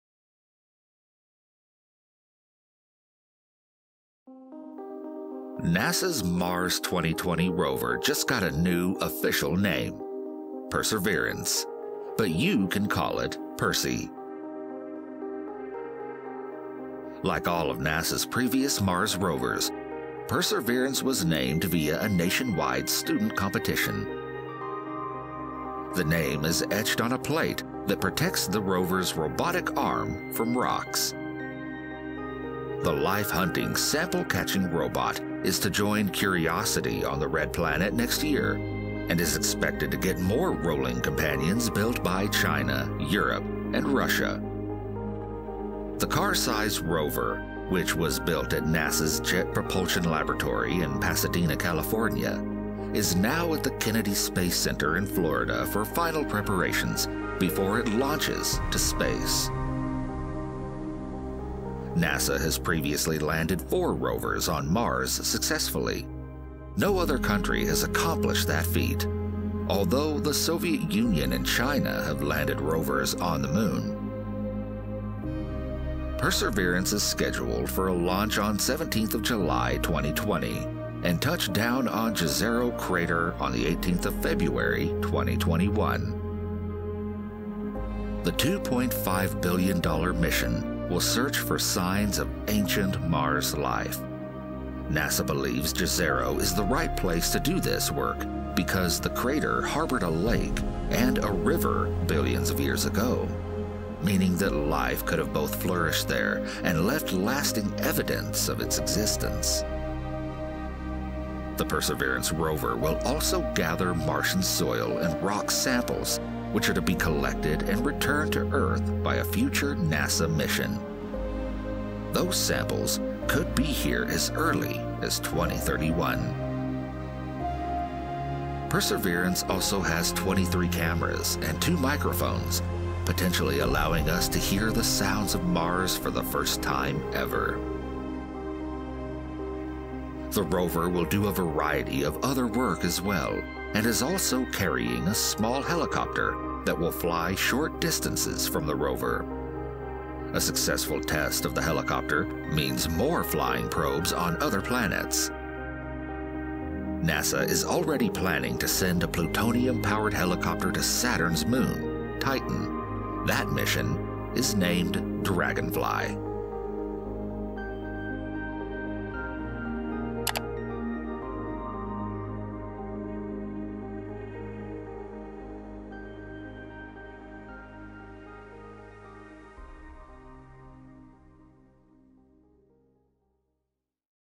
Meet Perseverance: NASA's Mars 2020 Rover
NASA's Mars 2020 rover just got a new, official name. Perseverance. But you can call it "Percy". The life-hunting robot is to join Curiosity on the red planet next year.

Narrator:
Russell Archey

Credits:
NASA/JPL-Caltech/ASU
NASA's Goddard Space Flight Center Conceptual Image Lab